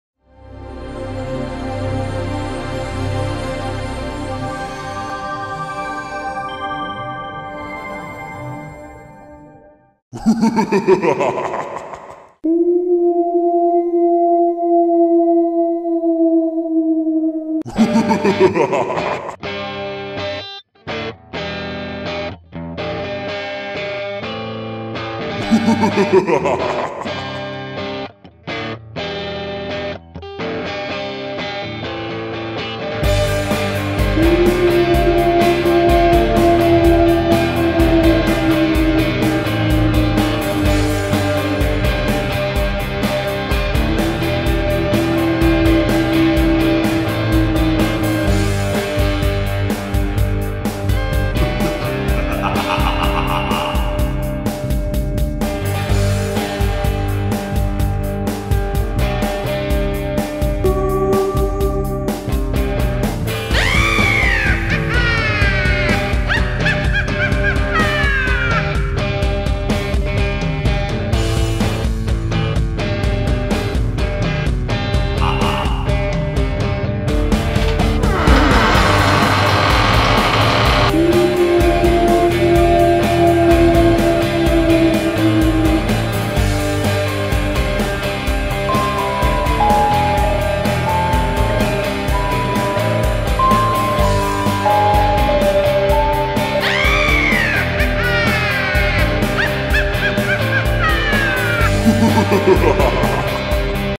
Happy Halloween!
CCLS Miami wishes all our students a very happy Halloween! Watch our video to learn about some Halloween traditions and decorations.